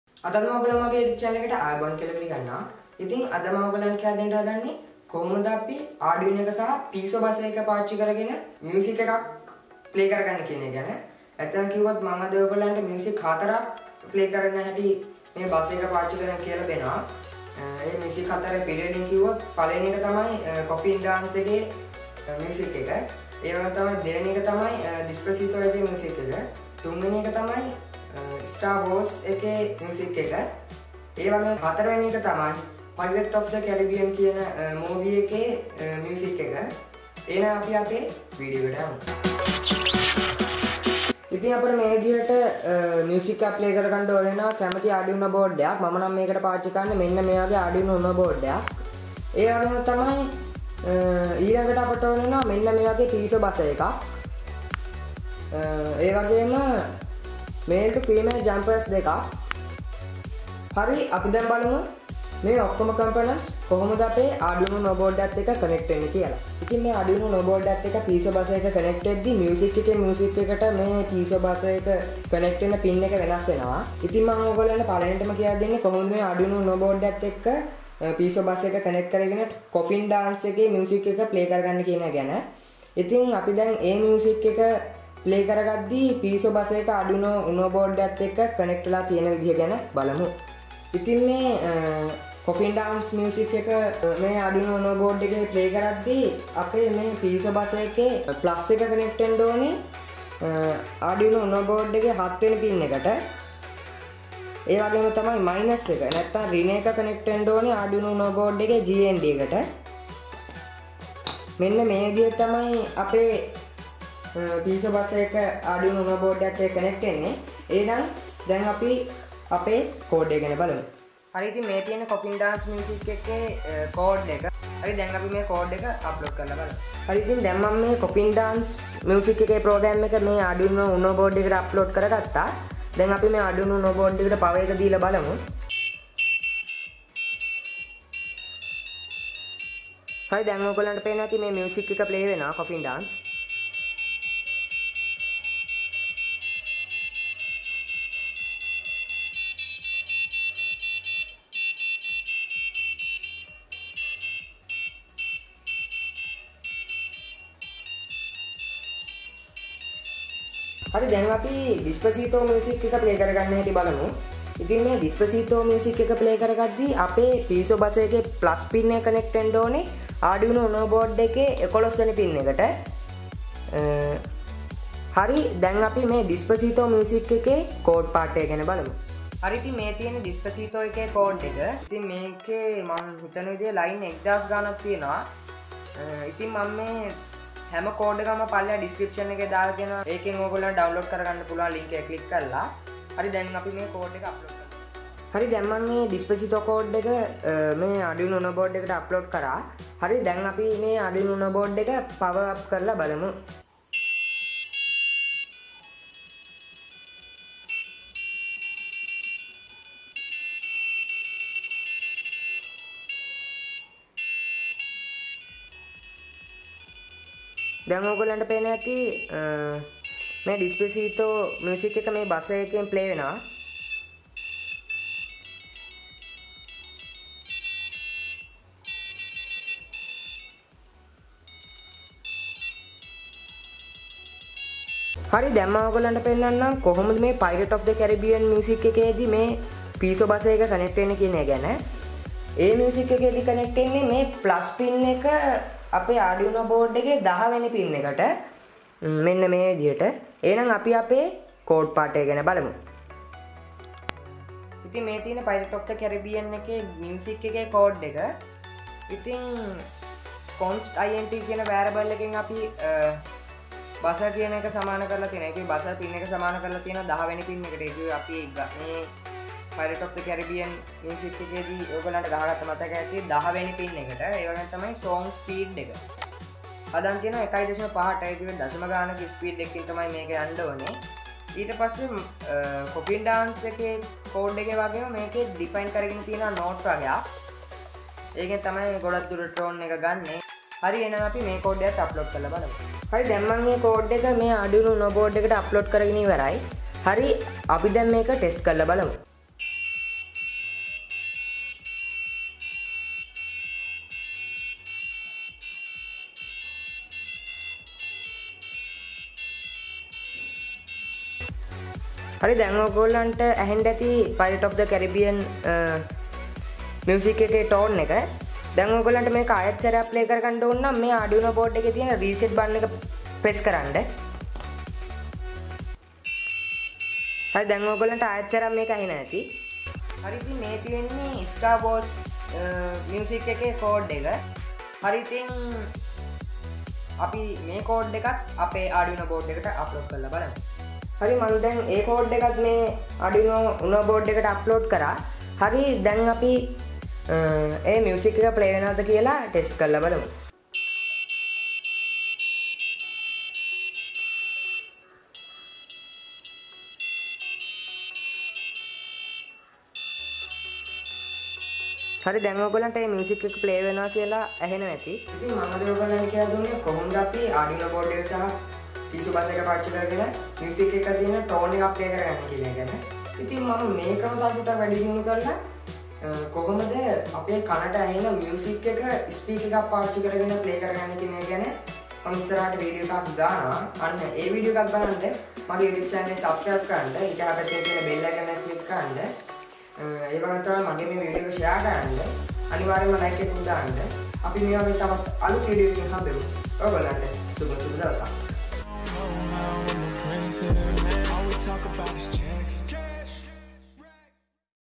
Sinhala Arduino | How to play music from Arduino
Components-
                       1)Arduino uno Board
                       2)Piezo Buzzer
                       3)Male to female jumper wires

Today i show you how to play Music using Arduino,
i  think Will be  this video is very important for you ,